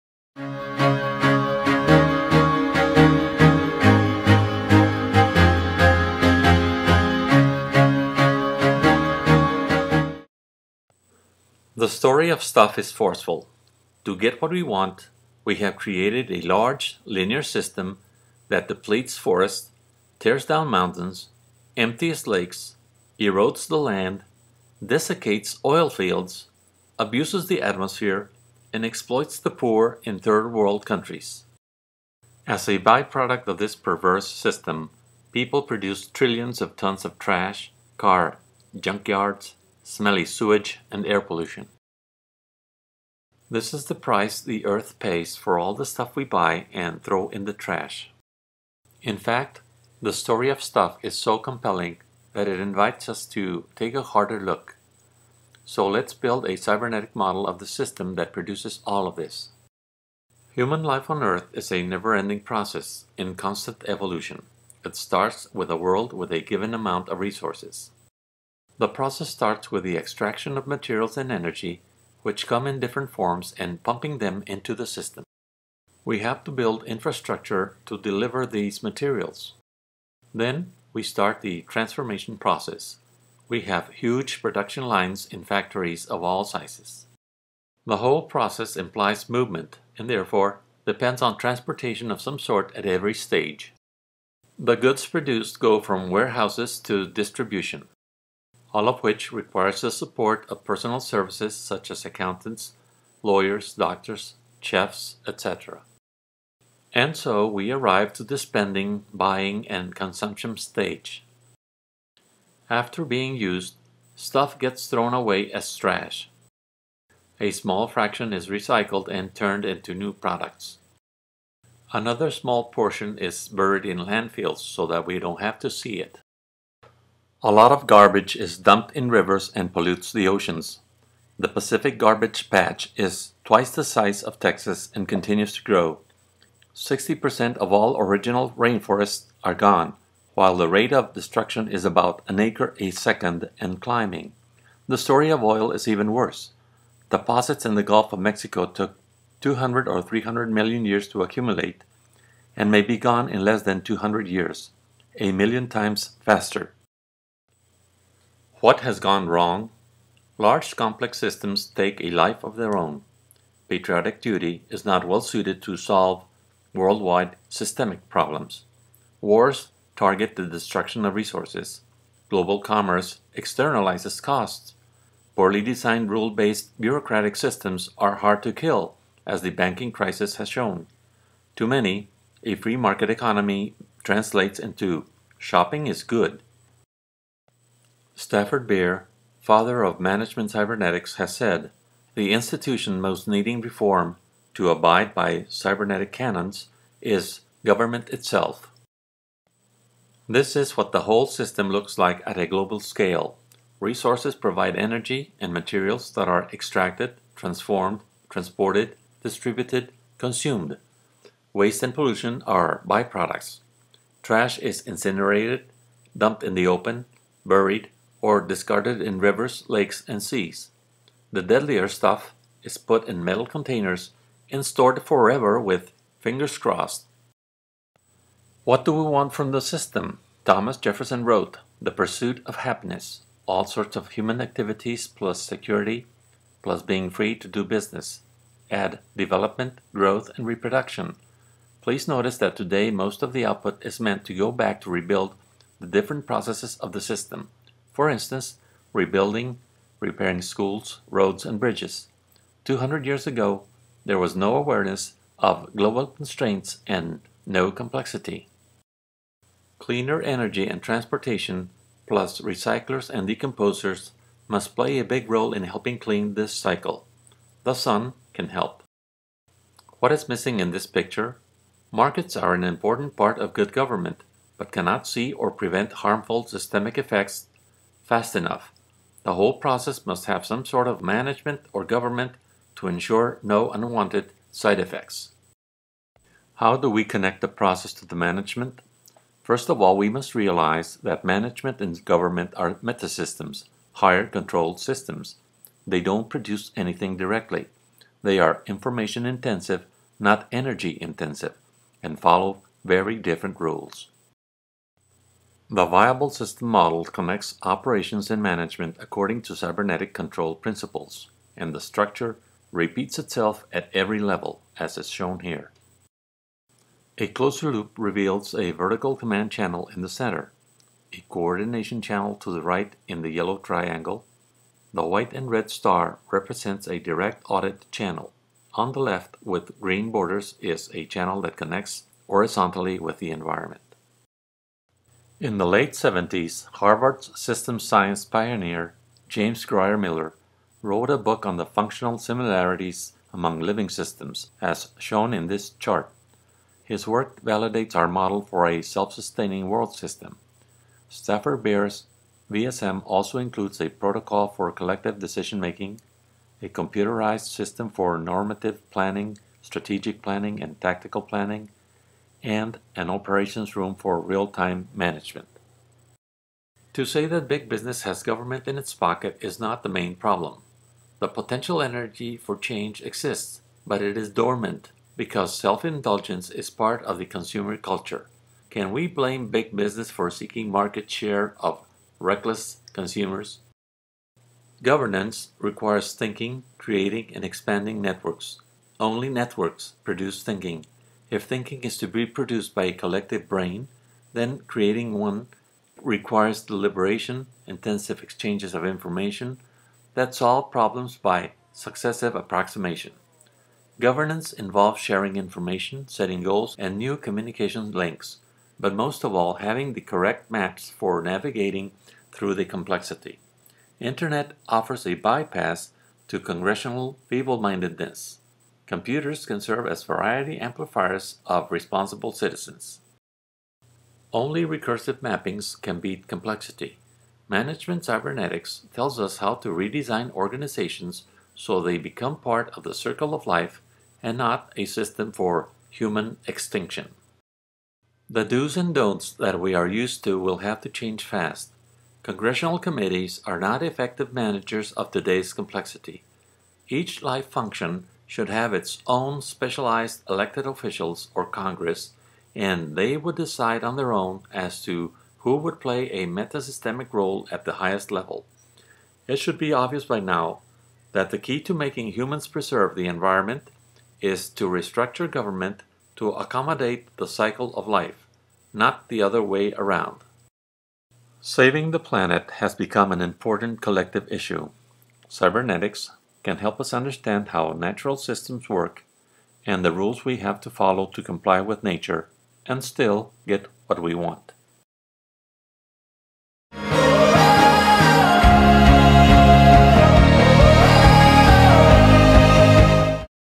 Stuff, Life & Cybernetics
The Story of Stuff is qute compelling so we use the Viable System Model and Management Cybernetics to take a hard look at the system that is producing so much waste and resource depletion in order to suggest important changes. Goverment will have to le redesigned to stop consumer madness and find sustainable alternatives. Markets are slow to react to behaviors that are destroying the planet's life-carrying capacity. We must act before it is too late.